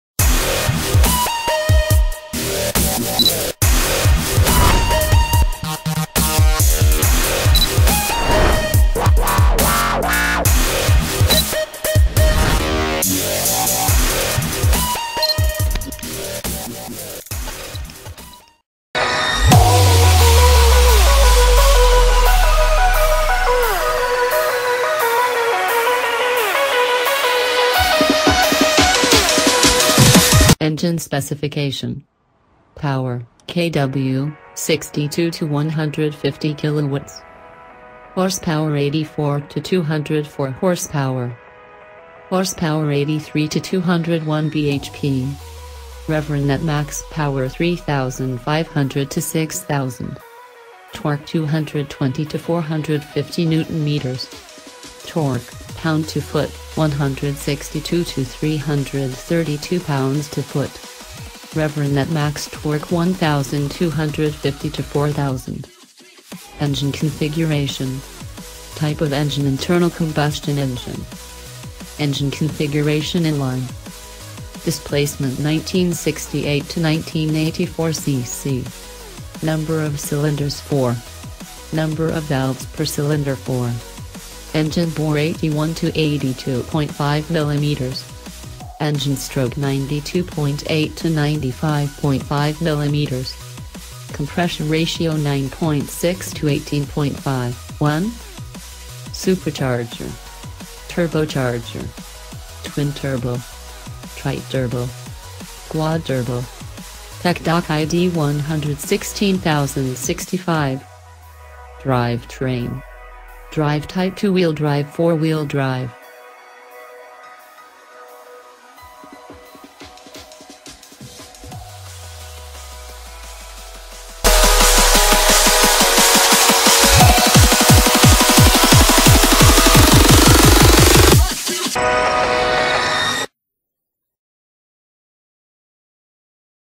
HOT NEWS !!! 2018 Volkswagen Multivan Exterior and Interior
HOT NEWS !!! 2018 Volkswagen Multivan   Exterior and Interior - Engine Specification

Power (kW)  62 – 150 kW
Horsepower  84 – 204 hp
Horsepower  83 – 201 bhp
Rev. at Max Power  3500 – 6000
Torque  220 – 450 Nm
Torque (lb-ft)  162 – 332 lb-ft
Rev. at Max Torque  1250 – 4000
Engine Configuration

$31,425

Popular Post !